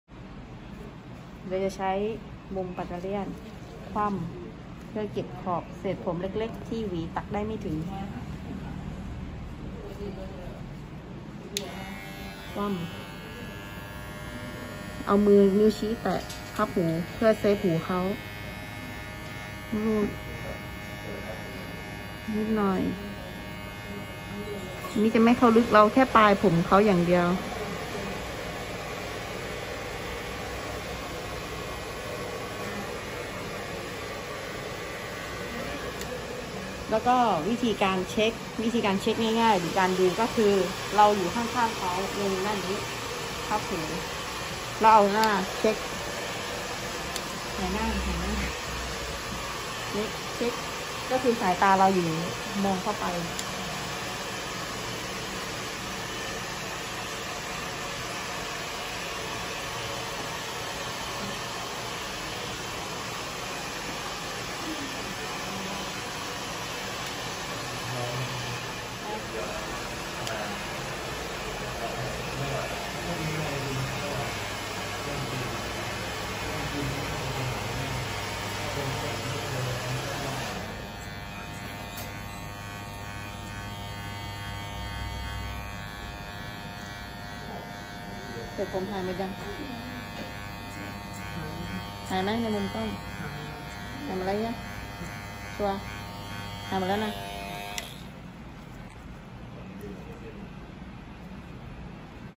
เทคนิคการคว่ำปัตตาเลี่ยนเพื่อตัดเก็บขอบผมเล็กๆ จากช่างเฟิร์น HRBB
💈 การเก็บรายละเอียดเล็กๆตรงขอบทรง ด้วยเทคนิคการคว่ำปัตตาเลี่ยน โดยช่างเฟิร์น Harisbarbershop

อุปกรณ์ที่ใช้ :
- ปัตตาเลี่ยนไร้สาย บาร์เบอร์ เบรน แบล็ค บีสท์ 2